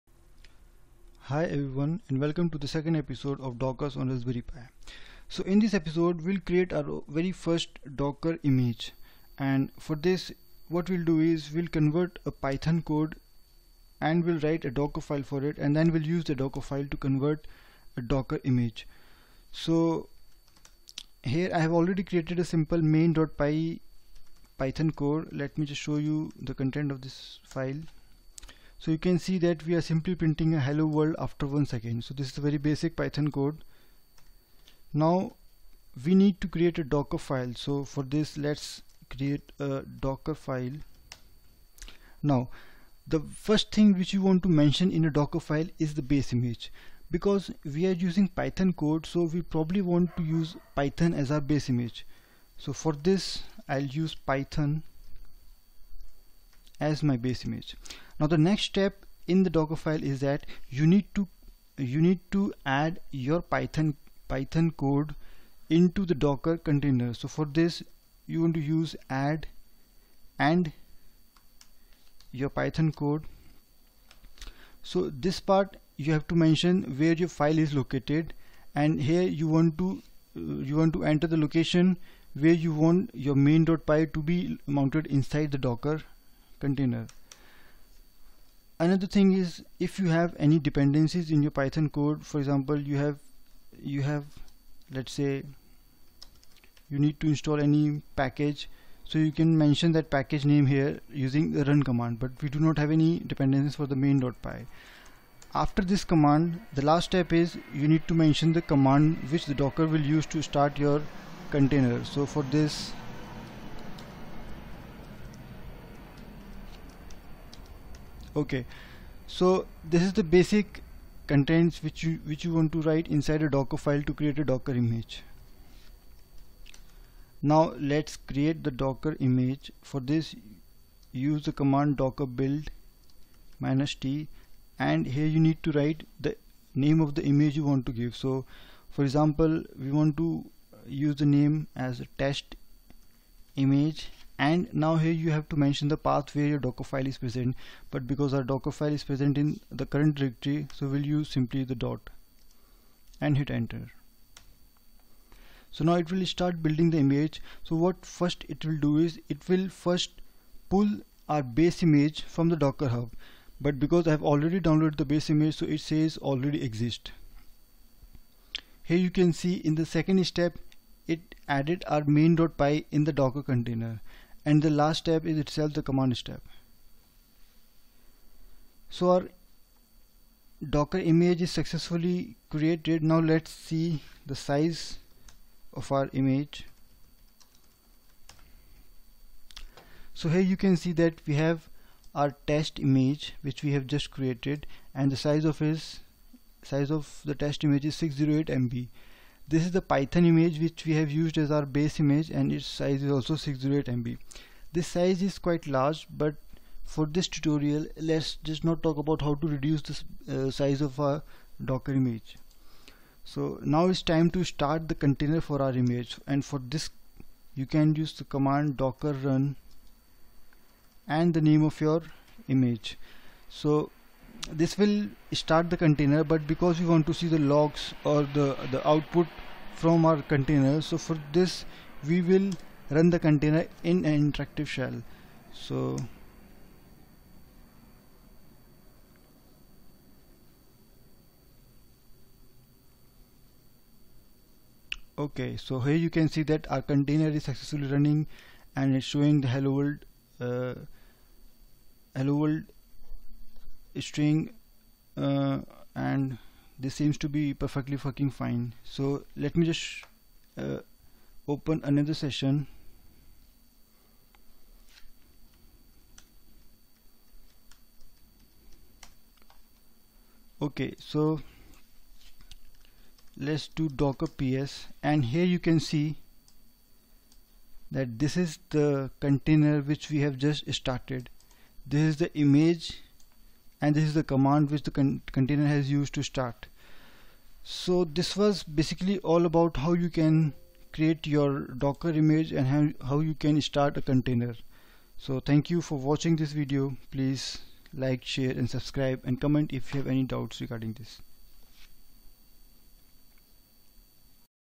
Docker on Raspberry Pi Ep.2 | Creating Docker Images
This video explains some basic concepts on creating a docker image.
Python code is used and is converted into a docker image.

Command to build image: docker build -t testimage .
Command to run container: docker run -it testimage